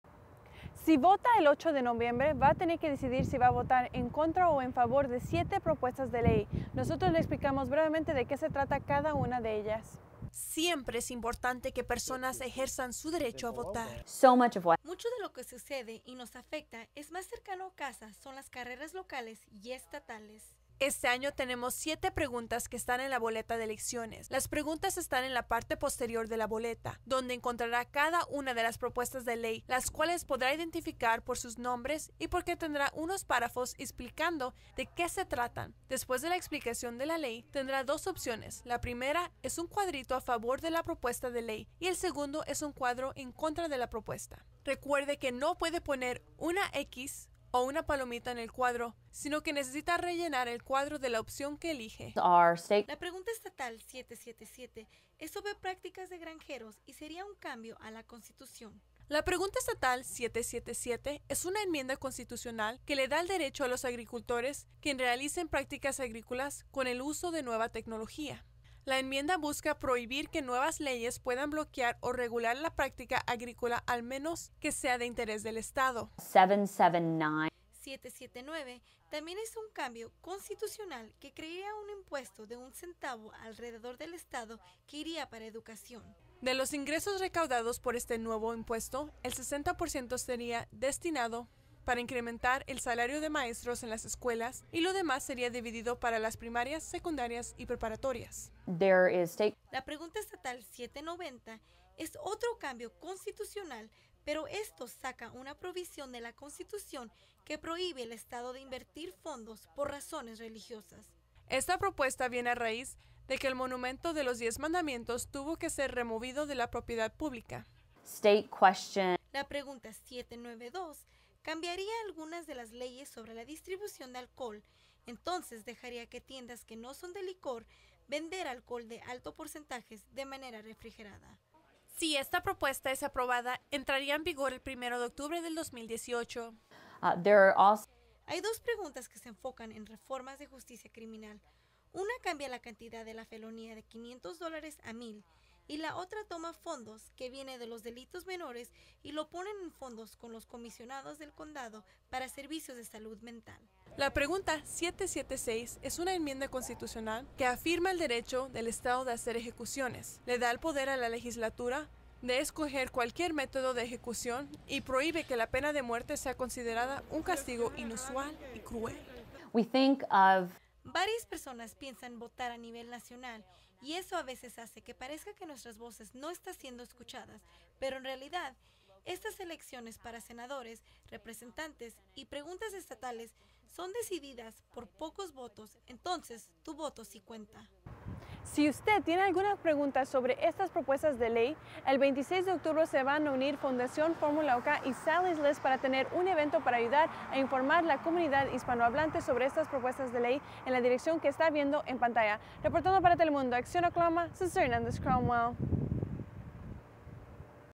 7 Propuestas de Leyes Estatales
Este año tenemos siete propuestas estatales en la boleta de elecciones.